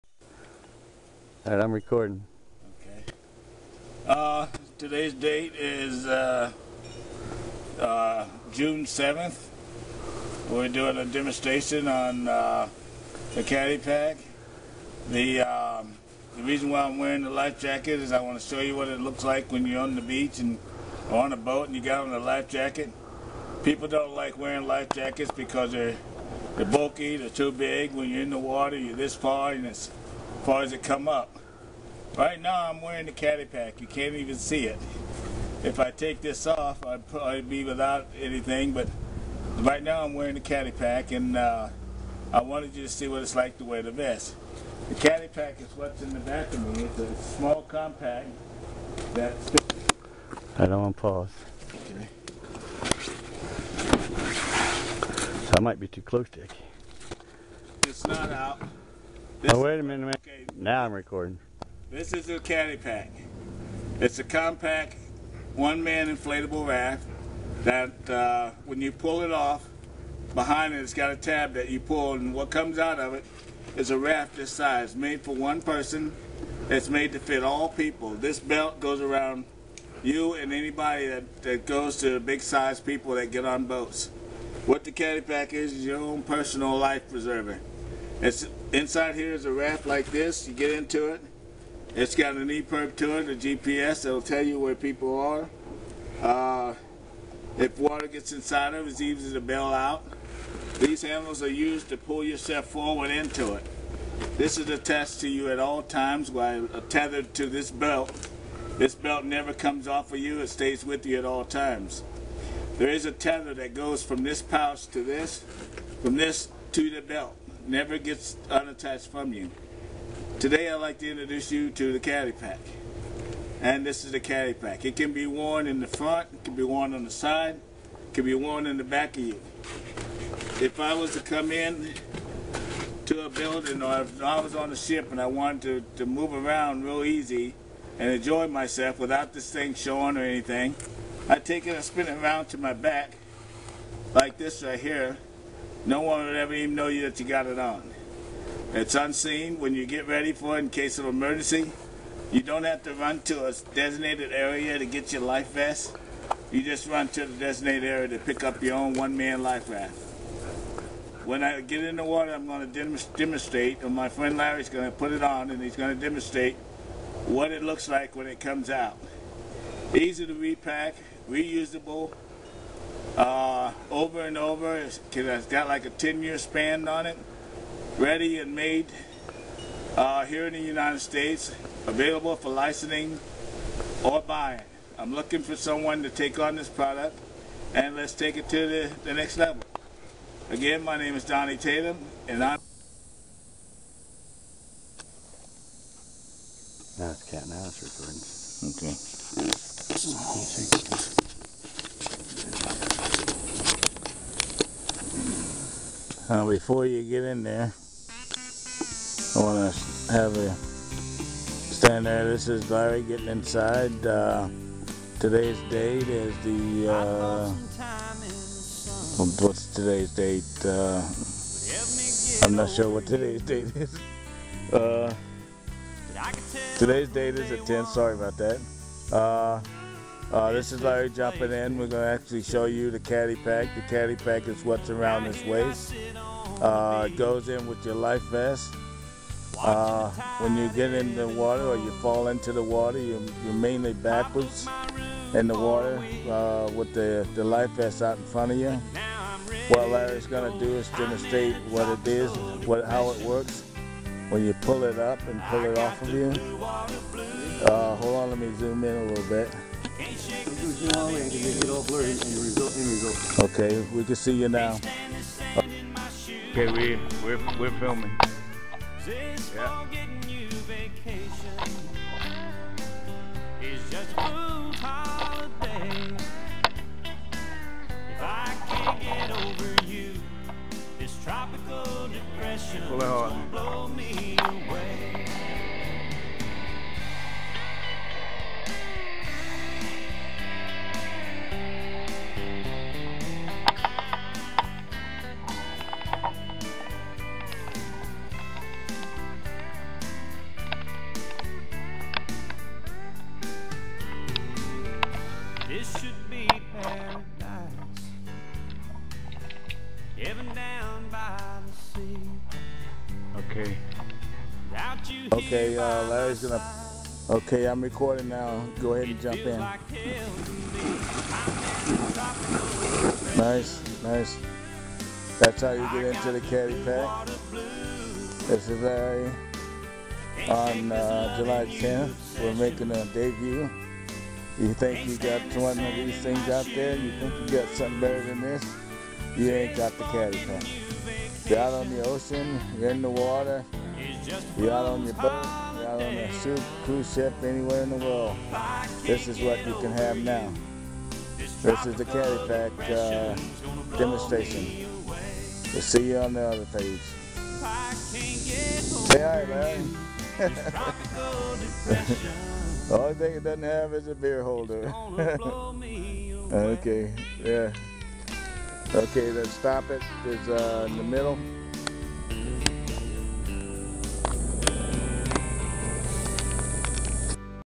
New invention "Wearable Life Raft " The Caddy Pac LLC
Manufactured for your safety on the waters inland or out to sea, the Caddy Pac is made with your safety and survival as it's number one priority.

The Caddy Pac is a wearable compact inflatable life raft created to be your own personal survival pack.

The Caddy Pac fits all size people and provides a safe and reliable way to
EXIT the water in case of emergency.

My name is Donnie Tatum and I am the inventor of the Caddy Pac a I believe this procuct will benefit all who fear for their safety in or around water. I am seeking a buyer for my product or a licensing agreement in 2013. This product can be used on airlines, cruise ships, oil rigs, pleasure boats,recreational vehicles,dive boats,single engine planes, etc.

Please help me bring unnecessary drowning to an end.